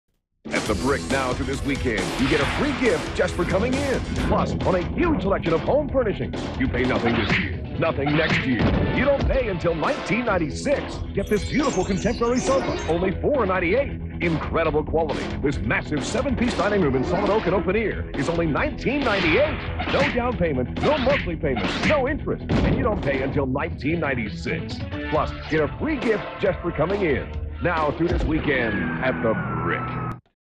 The Brick commercial from 1994
Pulled from VHS tape of Dec 18th 1994 broadcast of CITY TV (Toronto)